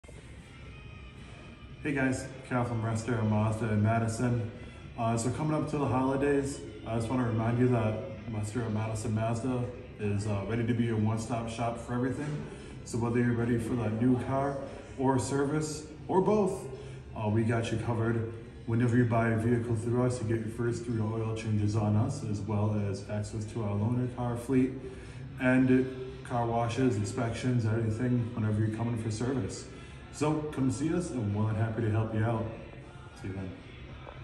Russ Darrow Madison Mazda Two For One
Service or sales? We have your back here at Russ Darrow Madison Mazda! With the holidays and winter coming up, we can help you get into the vehicle you need. Winter tires, oil changes, fluid checks, are just a few ways we can help you be winter ready.

Click the link here or in our bio to get you setup for a sales or service appointment today